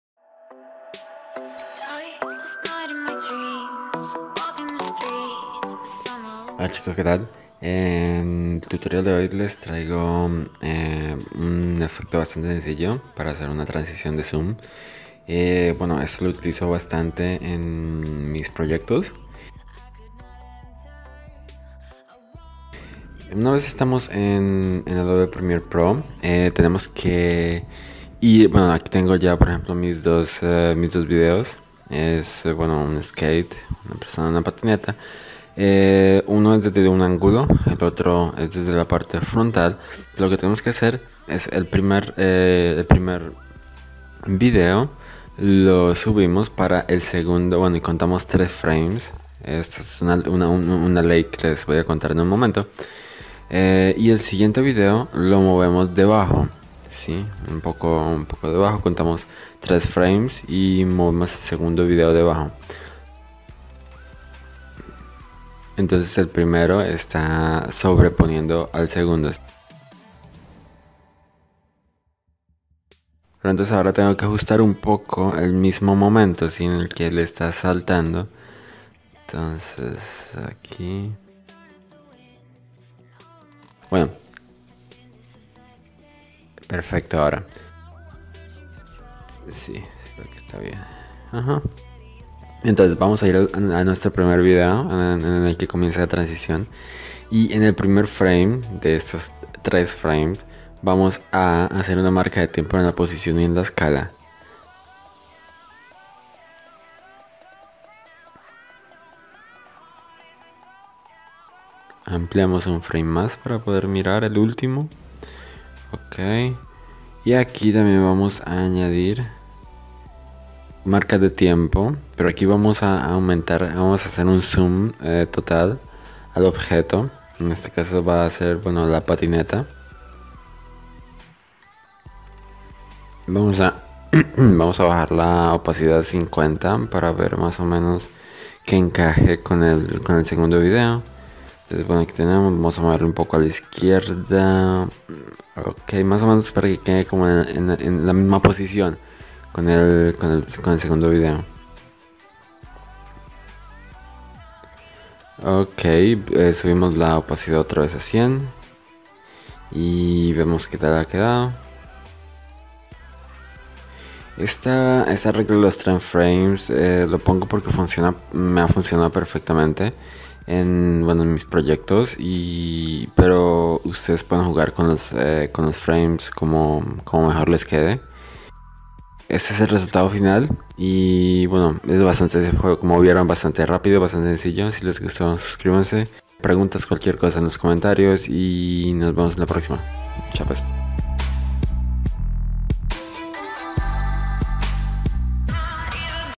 Tutorial: rapida transición de ZOOM en Adobe Premiere Pro CC 2017 en español
| - EFECTO de transición de ZOOM rapido en Adobe premiere pro cc 2017 facil y sin pluggings - | TUTORIAL

En este tutorial de edición de vídeo, te mostraré cómo realizar una transición con ZOOM in y ZOOM out solo usando marcas de tiempo. Esto es ideal para secuencias de efectos especiales en videos musicales u otros proyectos. Si tienes alguna pregunta dejarme saber en la sección de comentarios de abajo, ¡Gracias!
SUBSCRIBETE en Youtube para nuevos videos!